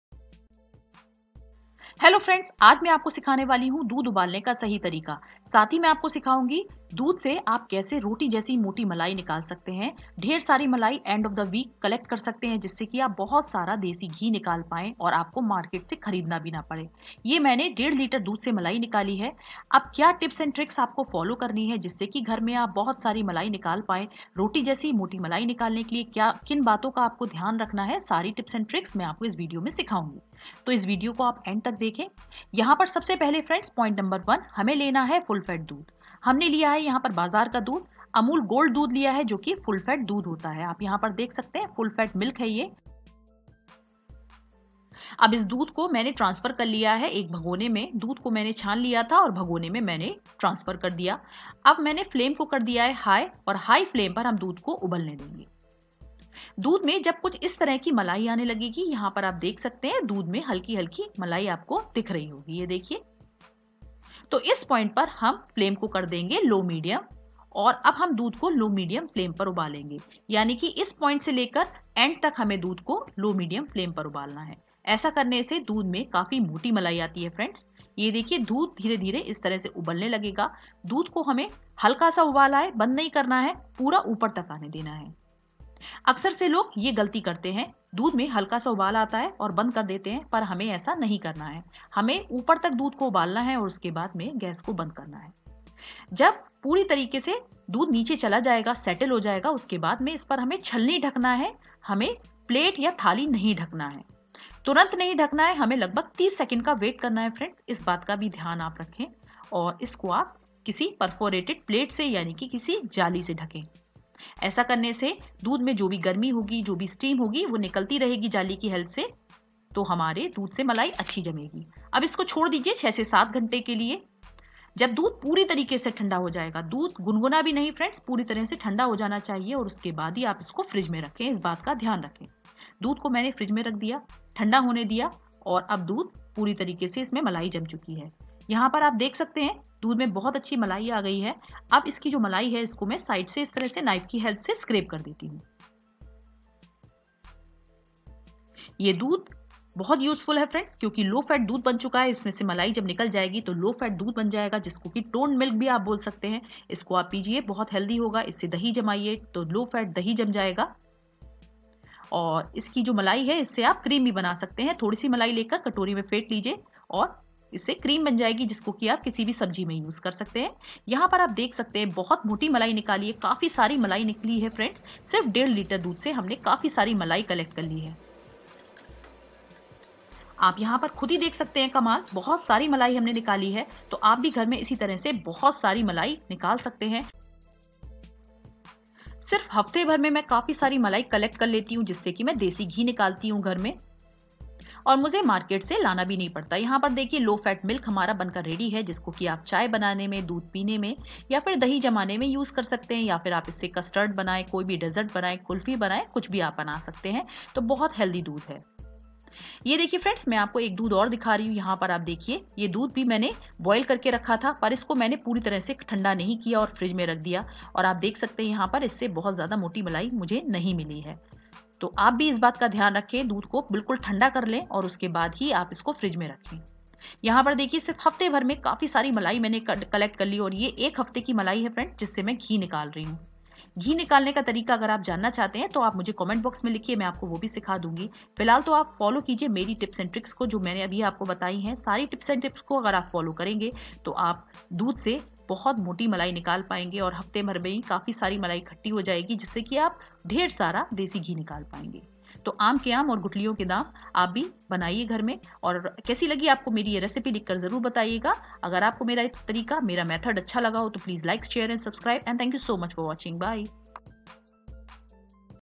दूध में रोटी से भी मोटी मलाई लाने की ये टिप्स-ट्रिक देखकर आप हैरान रह जायेंगे-How to boil Milk
दूध में रोटी से भी मोटी मलाई लाने की ये ट्रिक देखकर आप हैरान रह जायेंगे